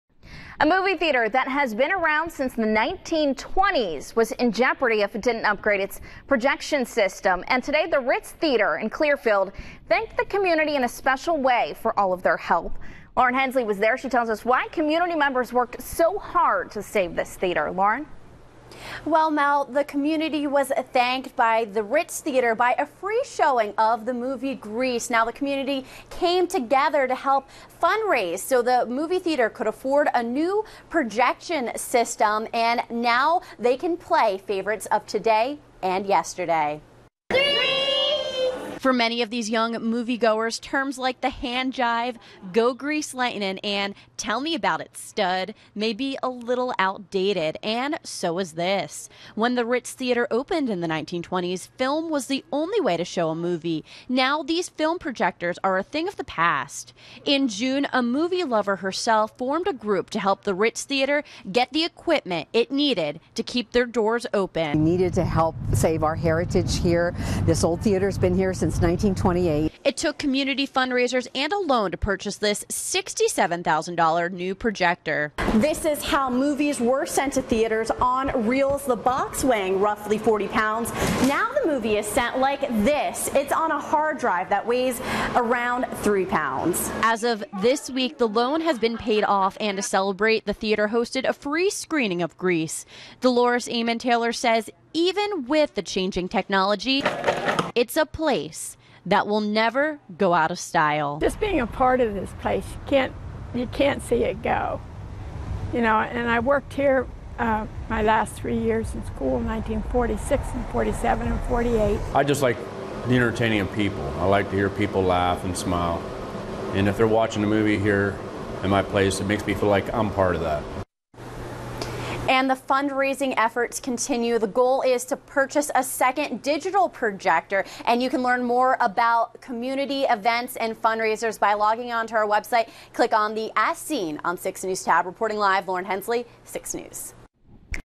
Ritz Theater pays off loan for new projection system
By: Lauren Hensley

CLEARFIELD, Pa. -- A Clearfield County movie theater that has been around since the 1920s was in jeopardy if it didn't upgrade its projection system. Sunday the Ritz Theater thanked the community in a special way for helping them raise enough money to purchase a new projector system.

When the Ritz Theater opened its doors in the 1920s film was the only way to show a movie.  Now film projectors are a thing of the past and the theater was faced with the expensive task of updating its equipment or shutting down.

In June a community group formed to help fundraising efforts to keep the theater running.

"We need to help save our heritage here. This old theater has been here since 1928," said Pamela Babick, chair of Save the Ritz.

It took the community group helping to fundraisers and a $30,000 loan from the Clearfield Revitalization Corp. to purchase a new digital projector for $67,000. The loan has been paid off and to celebrate the theater hosted a free screening of the movie "Grease." Movie goer Dolores Amon-Taylor said both the classic movie and the theater remind her of her childhood.

"Just being a part of this place, you can't see it go. And I worked here in 1946, 1947 and 1948," Amon-Taylor said.

Even with the changing technology, the Ritz's owner said it's a place that'll never go out of style.

"I just like the entertaining people. I just like to hear people laugh and smile. And if they are watching a movie here at my place it makes me feel like I am part of that," Ritz Theater owner Bob Knepp said.

Fundraising efforts are still underway to help the theater purchase a second digital projector.